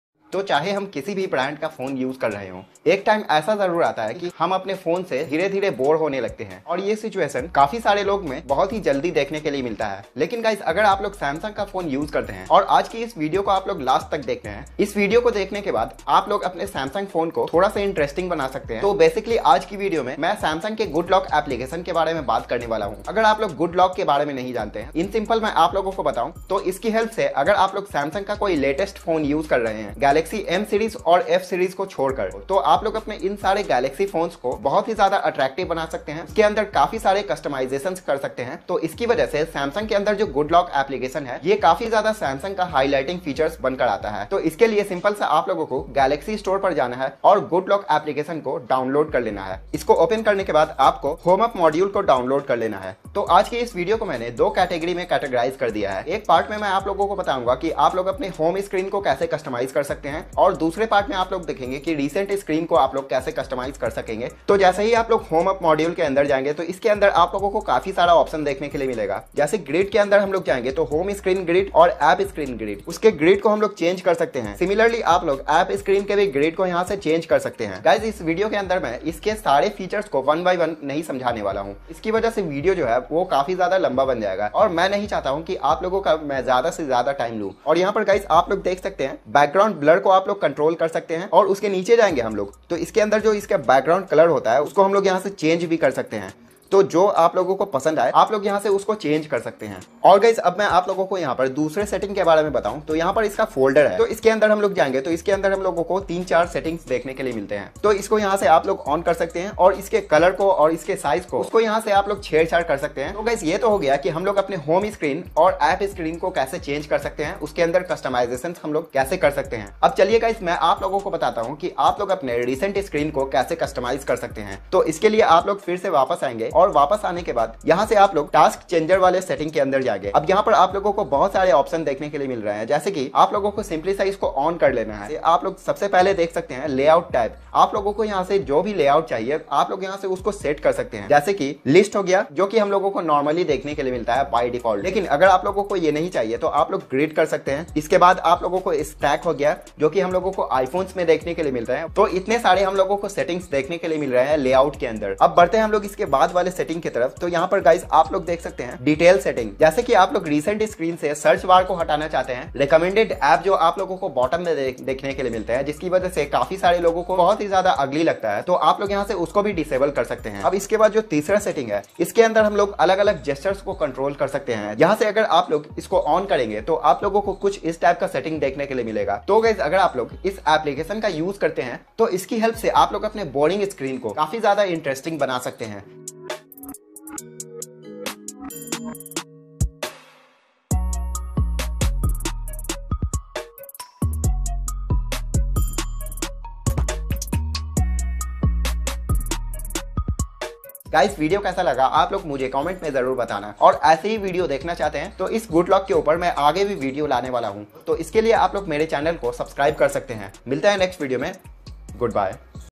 Samsung Goodlock "Homeup" Tutorial | How to Customize OneUI Homescreen | HomeScreen Customization
In this video I have explained to customize Home Screen of Samsung Smartphone,
       If the home screen of your samsung phone has become boring, then you should watch this video carefully because in this video I have explained how to make home screen attractive by using HomeUp module in Samsung's Goodlock app.